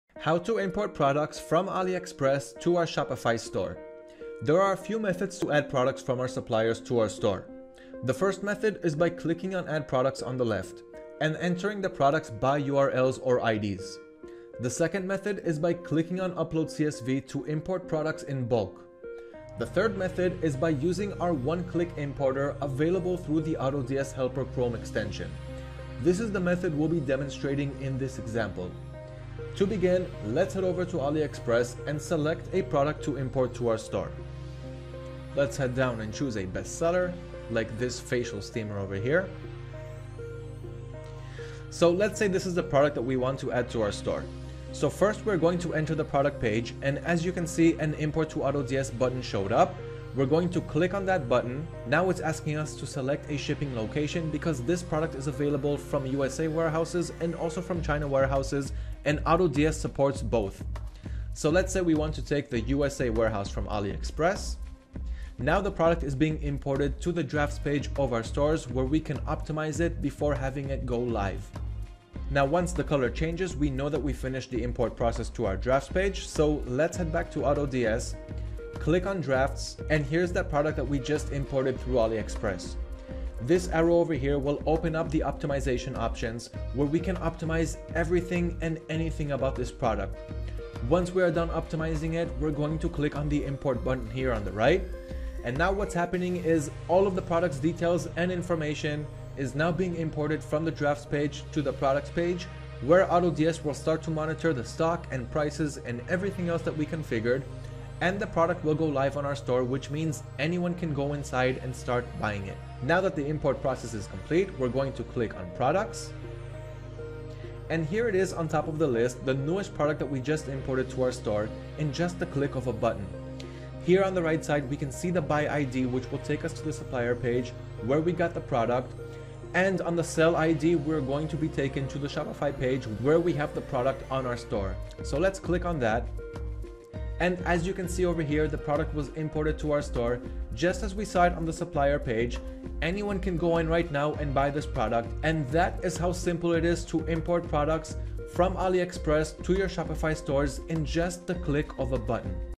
Import Products from AliExpress To Shopify
In this video we'll teach you a few different methods on how you can import products from AliExpress to your own Shopify store.

Please, DON’T click this link if you want to succeed with your business 👇

✅ Sell These Right Now - The HOTTEST Dropshipping Products